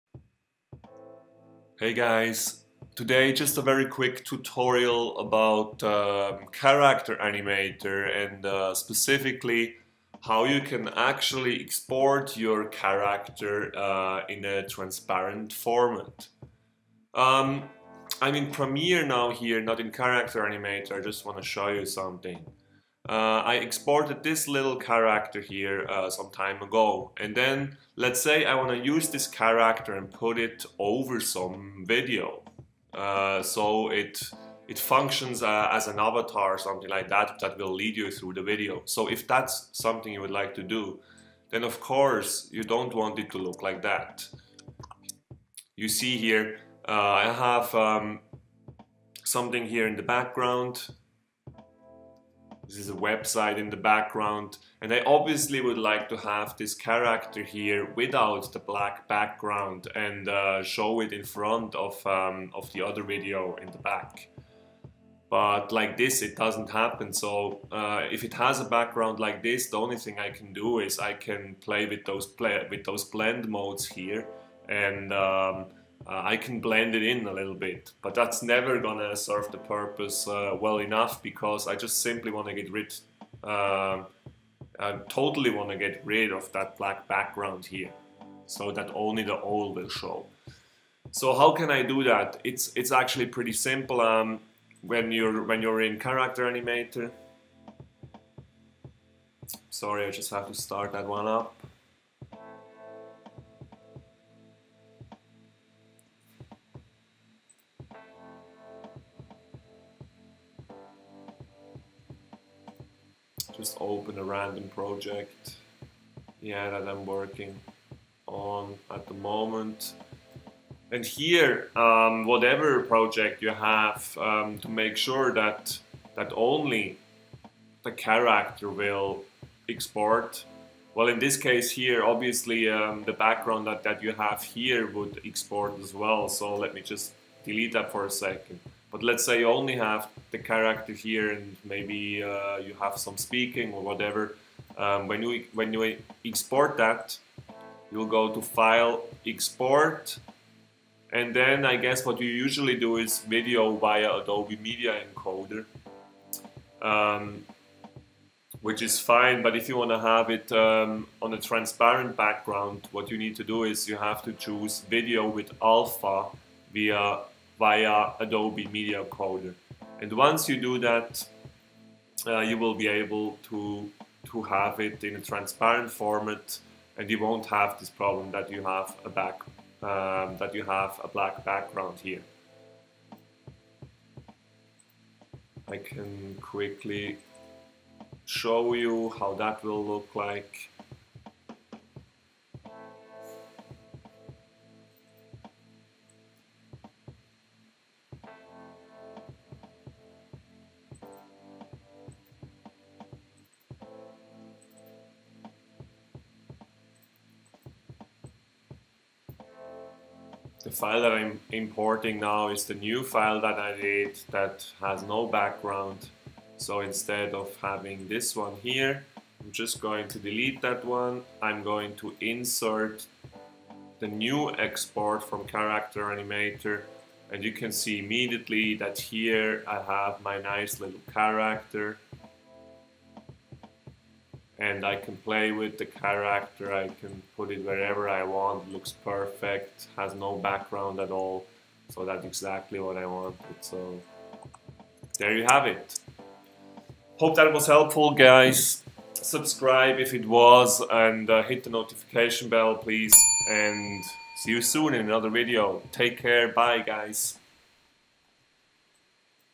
Character Animator: Export Puppet with a TRANSPARENT Background
If you would like to export your character (puppet) with a transparent background, so that you can use it on top of another video for instance (for example if you would like to use it as an avatar that guides people through a tutorial video that runs in the background), then use the method as described in this video.